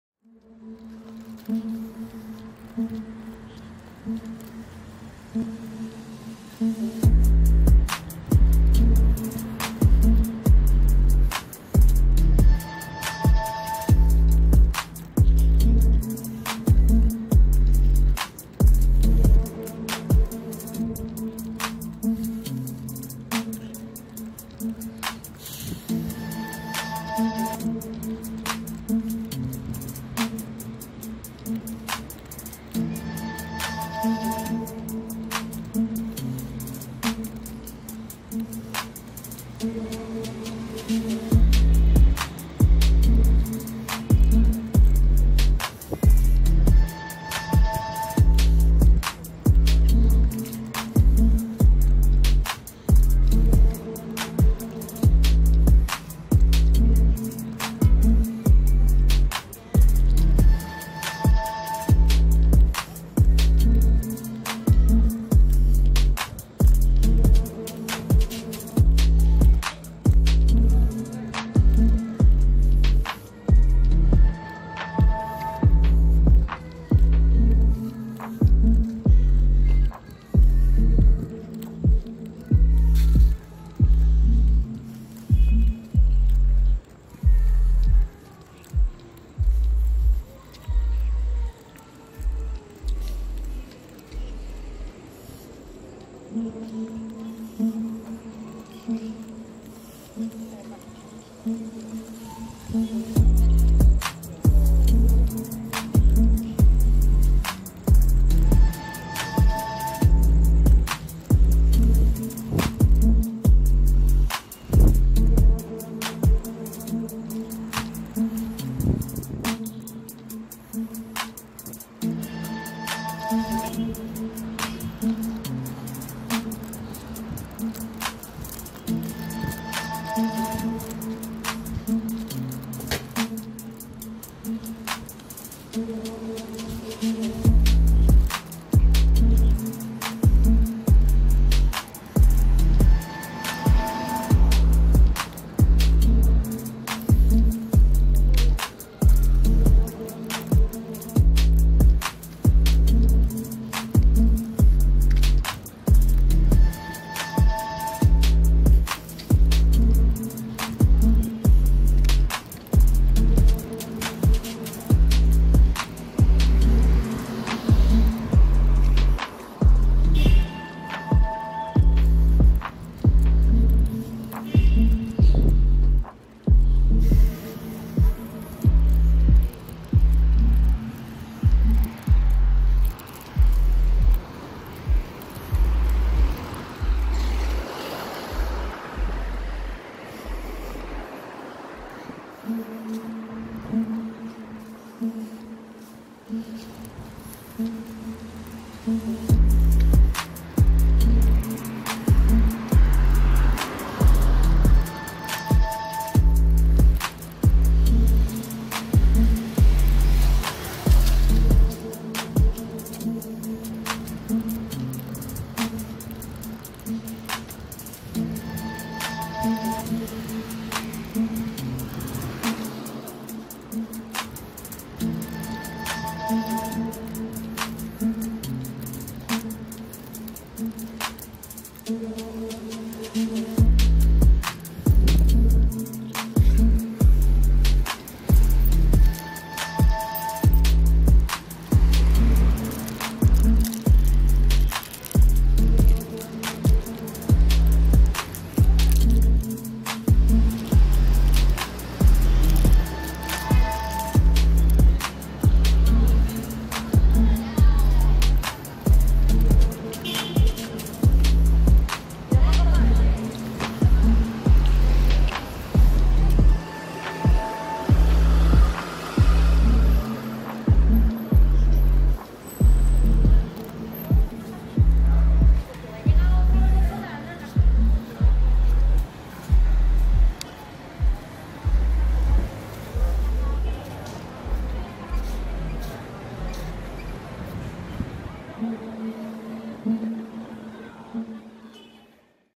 الاكوادور شوارع غواياكيل بالليل Roam the streets of Guayaquil at night
streets

Roam the streets of Guayaquil at night

في شوارع مدينة غواياكيل امريكا
 الجنوبيه سنتر مالكون